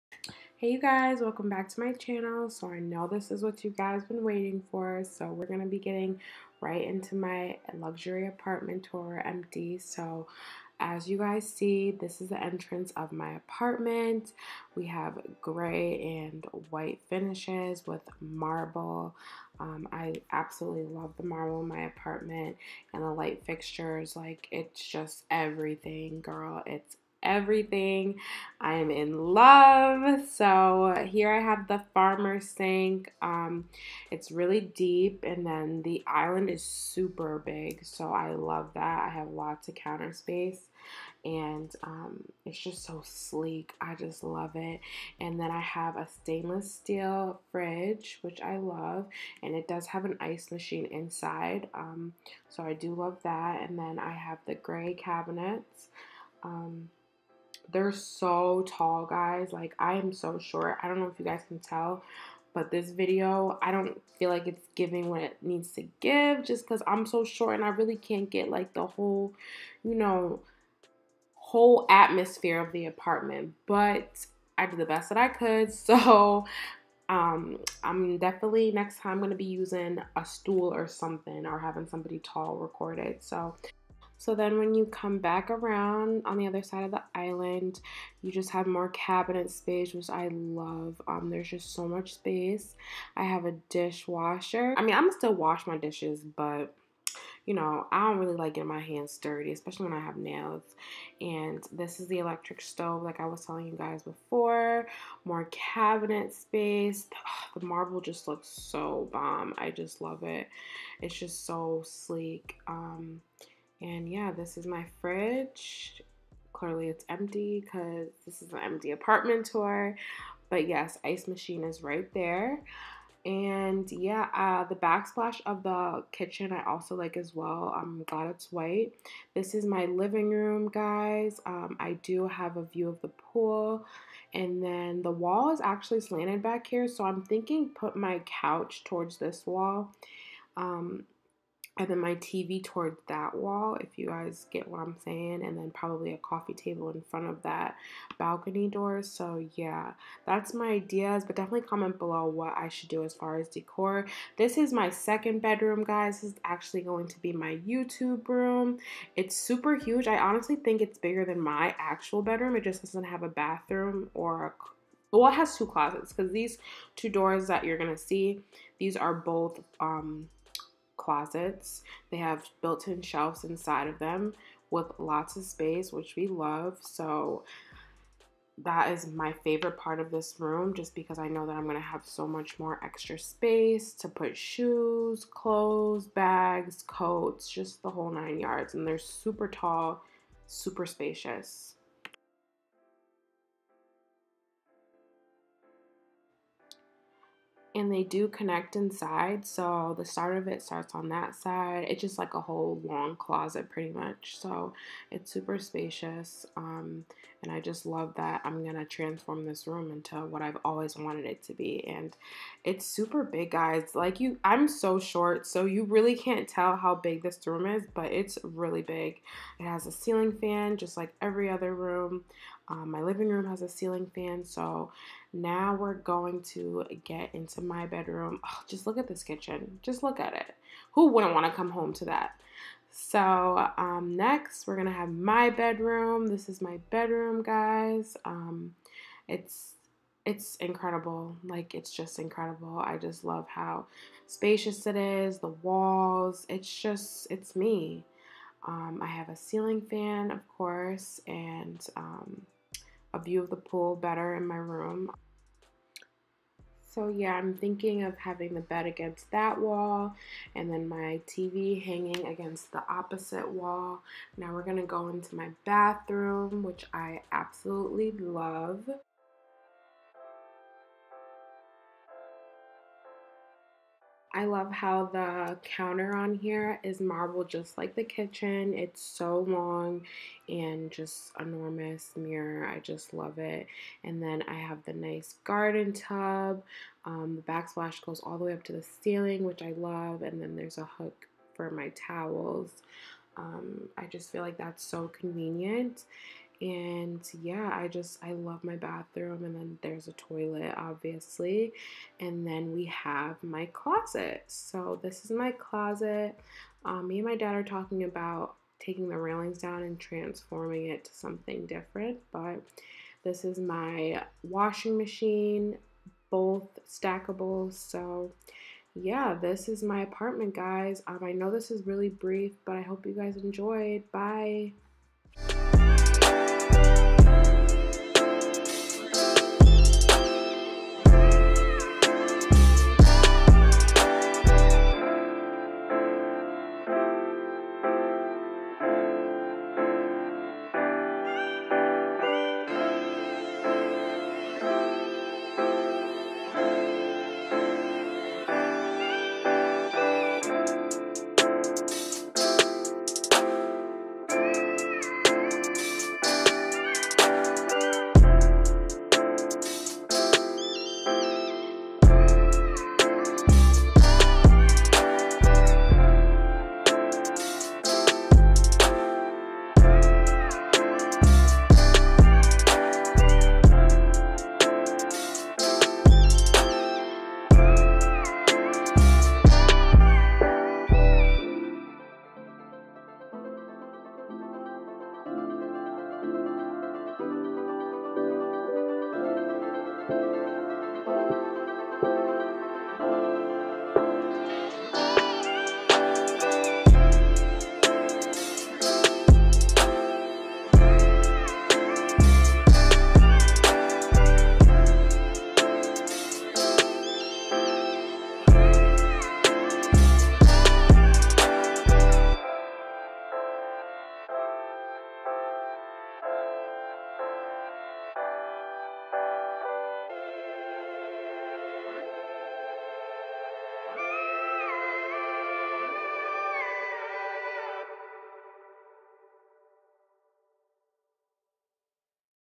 I MOVED TO ATL!! | MY EMPTY LUXURY APARTMENT TOUR
I hope you guys enjoyed this video I KNOW it was short but I was so overwhelmed when I filmed this..  but to those who know how hard I worked to get here, in this position, thank you so much for supporting me  there is so much more to come !!

Follow me♡:
lash ig: the.fairylashmuvaa
snap: lady_aquafina